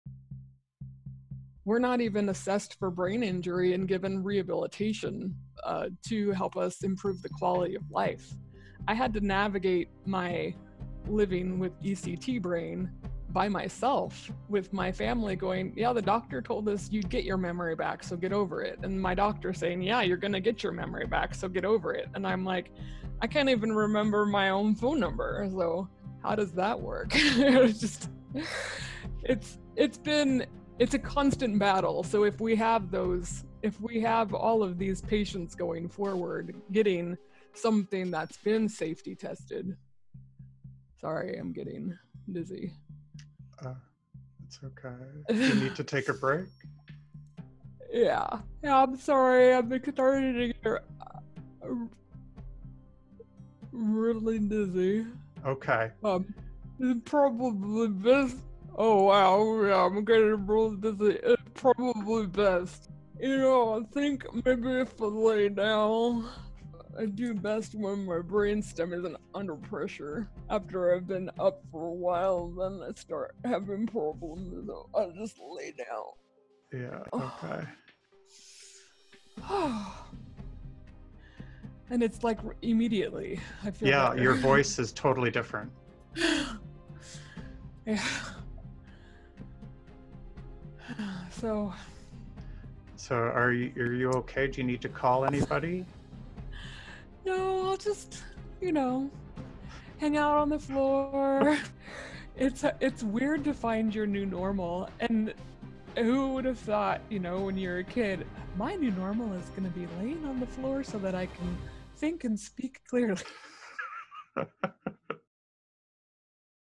Sarah Price Hancock trailer part 1: Surviving Shock Therapy (ECT) and psychiatric meds
This is not how I imagined the interview with Sarah would end. Listen to how Sarah's voice changes when she gets dizzy - and then changes again when she's on the floor.